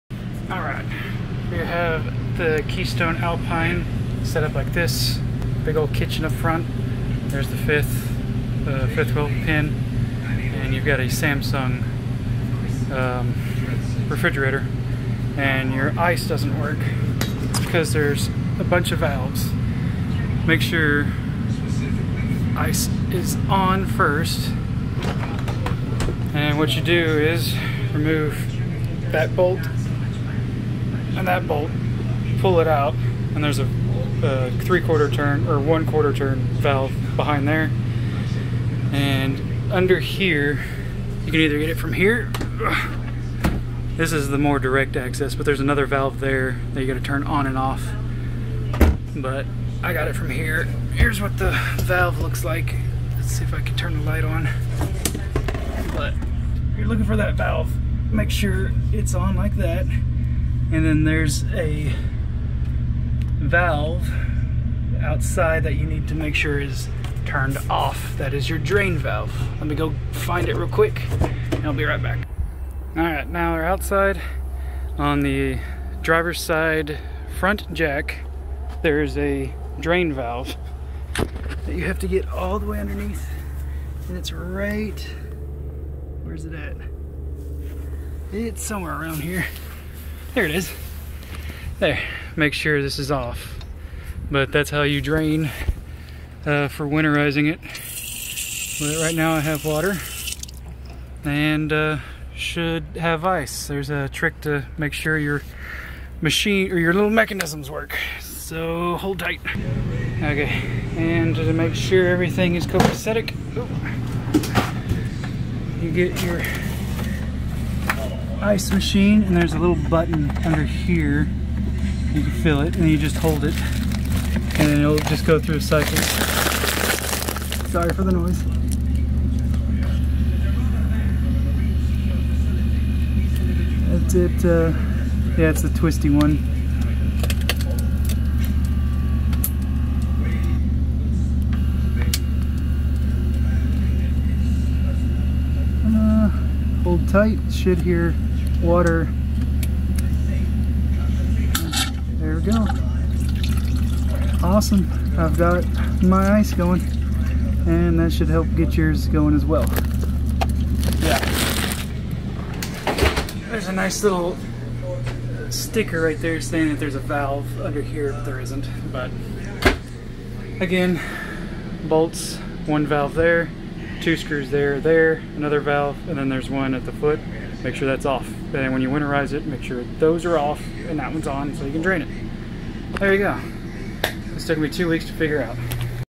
How to get water to Samsung residencial refrigerator ice maker. 2020 Keystone Alpine 3801FK
hope this video helps with setting up your trailer. I did all the hard work (the dealer should have done for me) and found where the valves are if you are having the same issue.

Cheers!